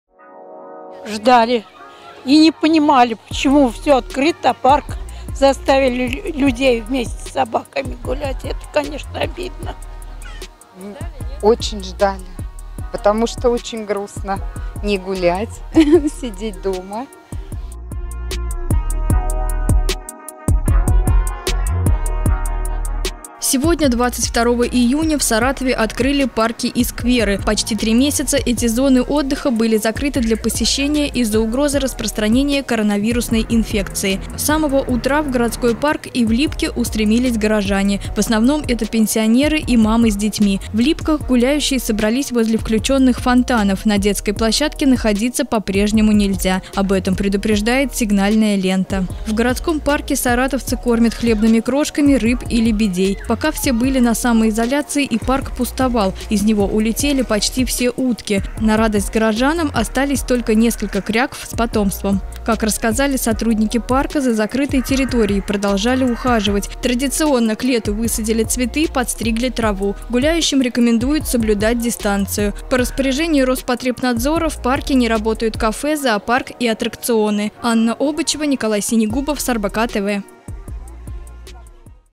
Саратовцы отправились на прогулку в открытые парки
Сегодня. 22 июня, в Саратове открыли парки и скверы. Почти три месяца эти зоны отдыха были закрыты для посещения из-за угрозы распространения коронавирусной инфекции. С самого утра в городской парк и в Липки устремились горожане.